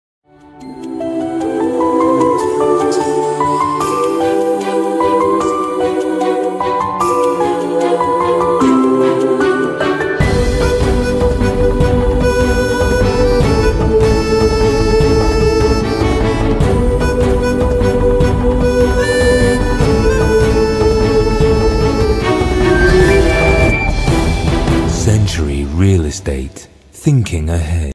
Work in Progress: Century Ethos
Century Ethos - Homes that express your style are beautifully refined residencies in the making in the heart of Bangalore, Hebbal. Century Real Estate Holdings Pvt. Ltd presents to you BDA approved ultra-luxury apartments. Gift yourself a lavish lifestyle. for more details.